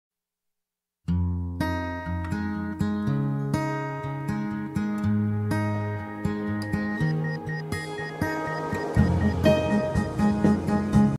Espadin Forest Pioneer 67 cm, piñon 11-13 dientes
Muestra mediante un traveling un espadin para procesadora marca Forest Pioneer 67 cm. Forest pioneer sawbar 67 cm,11-13 teeth.